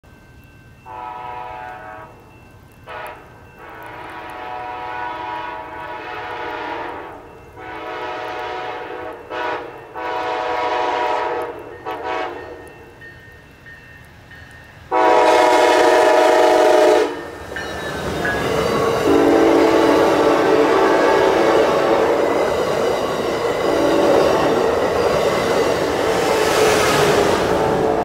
12/30/20 Amtrak 365
12/30/20 09:17 33 rain soaked degrees at CN MP 202 Charlotte as Amtrak 365 runs by on schedule for Battle Creek. Full 0:28 video.